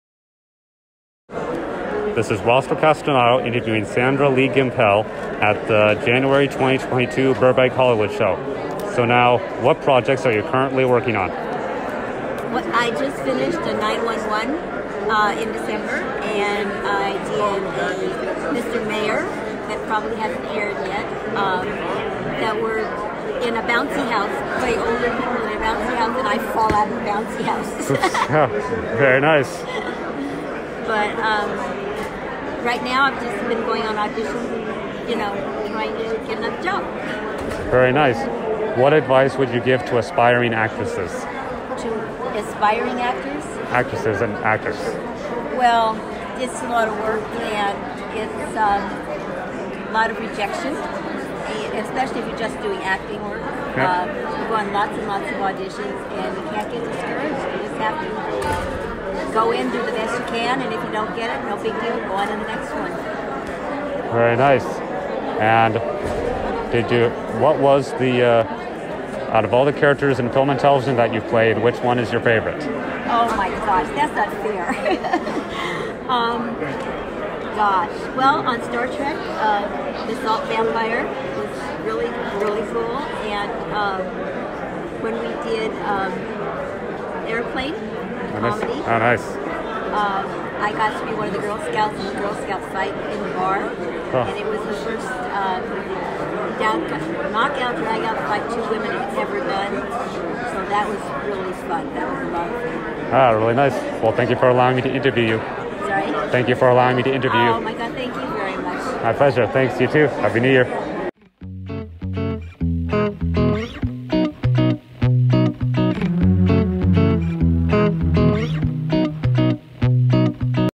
Sandra Lee Gimpel interview
Stunt double and actress, who was a stunt double for former child actor and actor Bill Mumy on the television series "Lost in Space" from 1966 to 1968 and who also did stunts on films like the 1996 Holiday/comedy film "Jingle All the Way," and who was also a stunt double or stand-in double for the late legendary comedienne and actress Betty White on the television series "My Name is Earl" from 2005 to 2006, and who also has appeared in films like the 2002 comedy film "Big Fat Liar." The proceedings or the donations that were paid for the autographs of her and Betty White were given or donated to Betty White's animal charity organization to partially honor what would have been her 100th birthday, on January 17th, 2022.